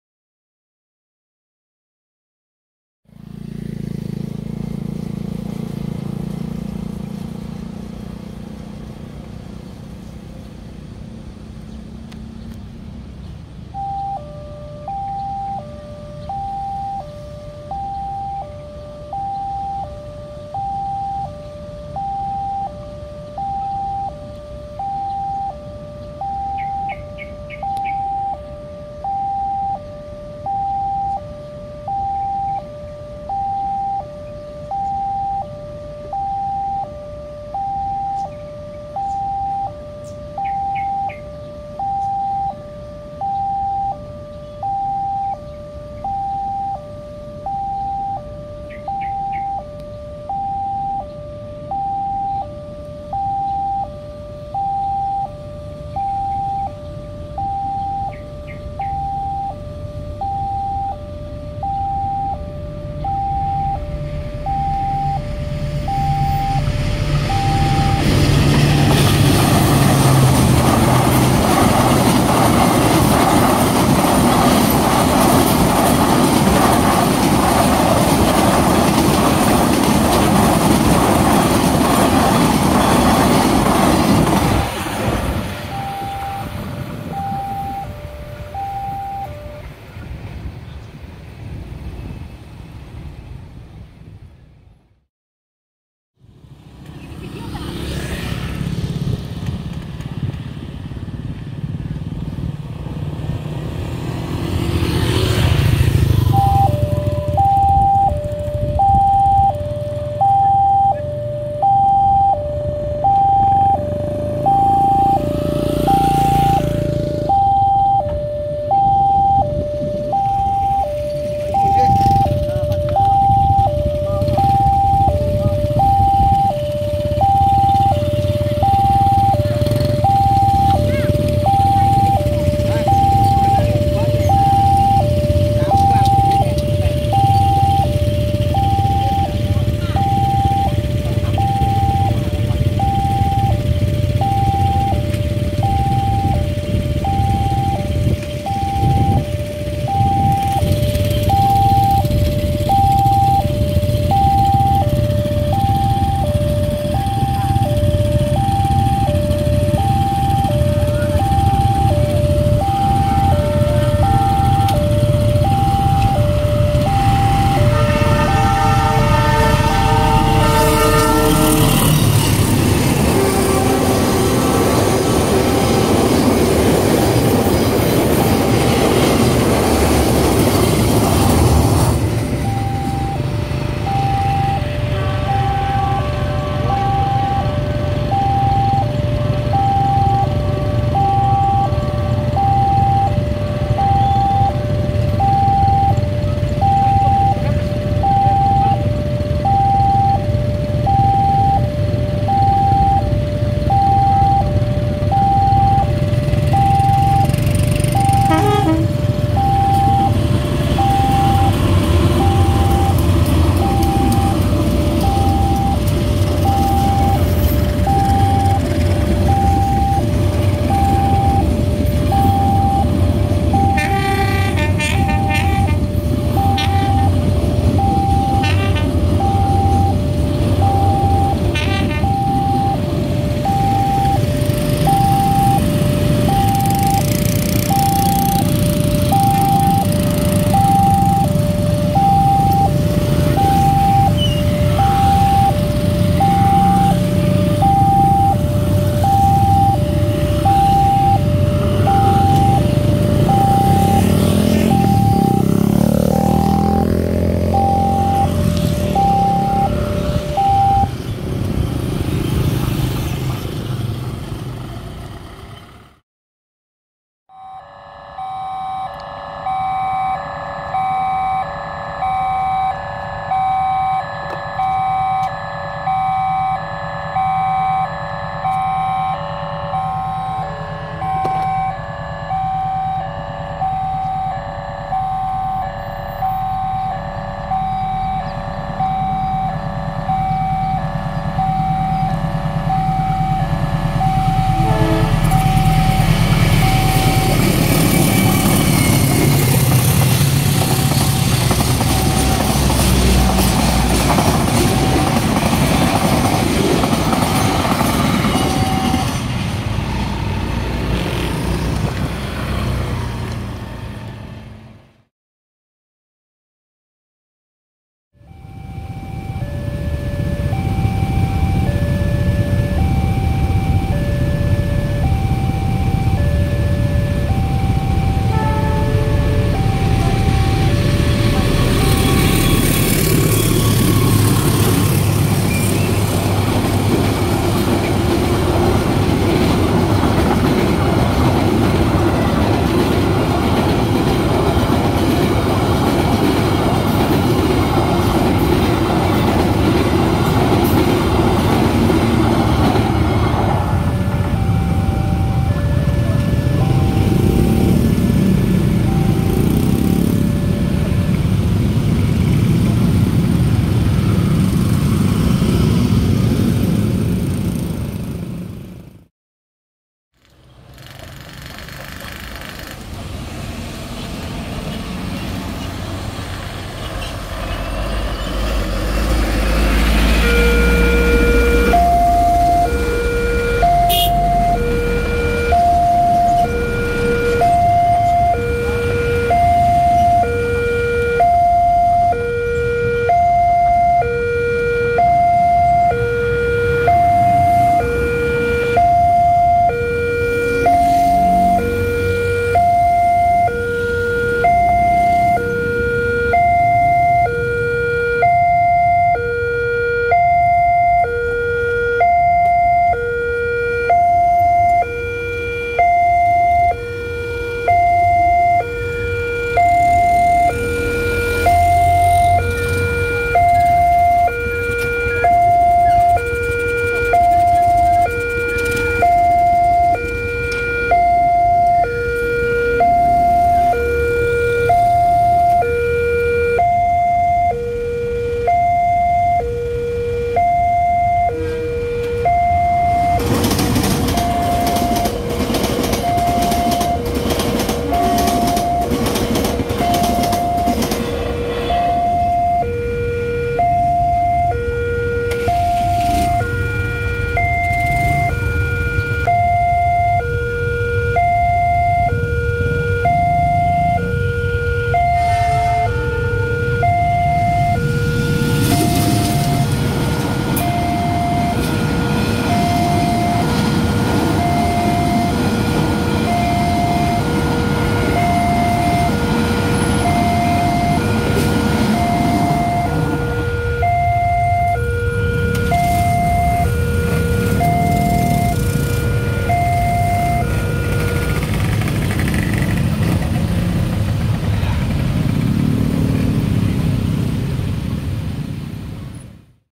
3 Perlintasan Kereta Api Otomatis | ALKMAAR x VIALIS | Palang KAB Rusak
3 Perlintasan Kereta Api dengan EWS (lampu & sirine) Otomatis yang ada di Kabupaten Indramayu, Jawa Barat
| Dengan merk ALKMAAR (lampu) x VIALIS (speaker)
| Palang KAB (Karya Anak Bangsa) yang Rusak sudah 5 tahun di JPL 157 Kertasemaya semoga bisa secepatnya diperbaiki
| sisanya hunting random ke JPL 174 & 175 Stasiun Arjawinangun, Cirebon